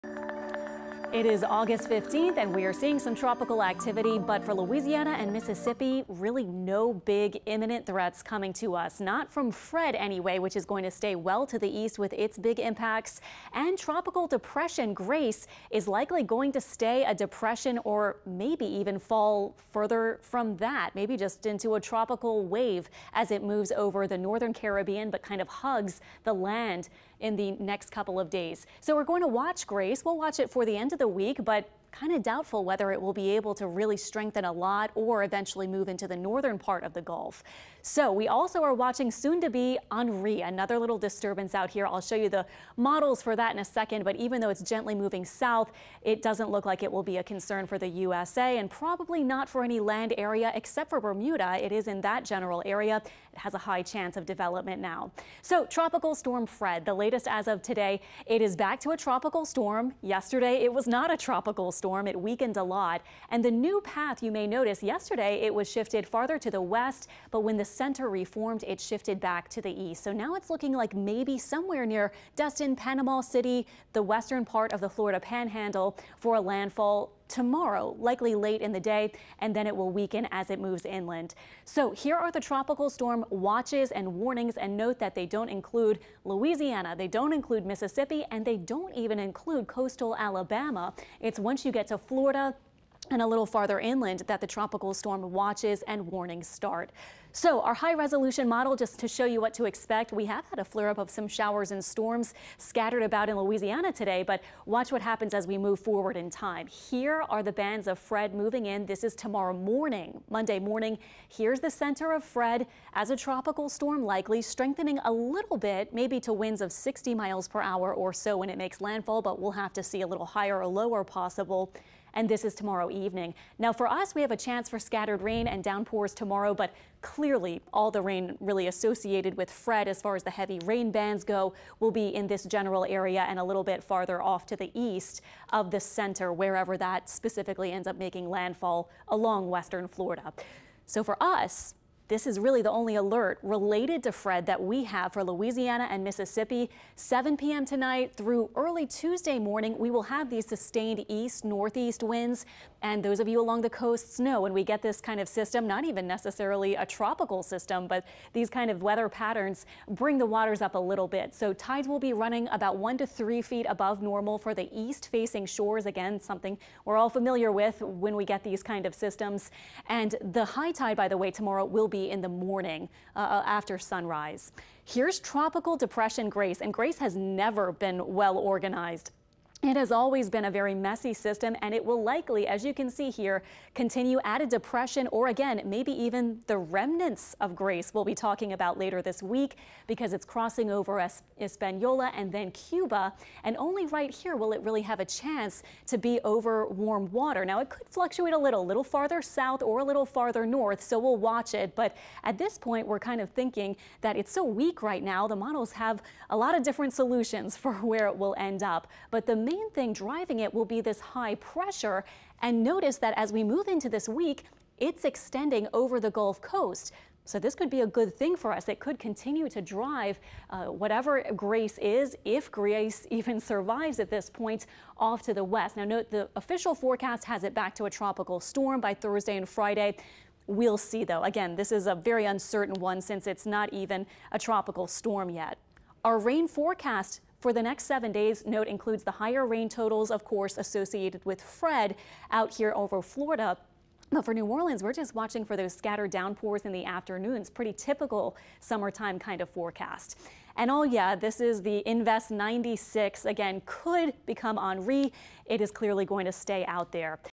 Sunday 5:30 PM Tropics update: Tropical Depression Grace and Tropical Storm Fred
Weather Expert Alexandra Cranford breaks down the activity in the Tropics as a new development comes in.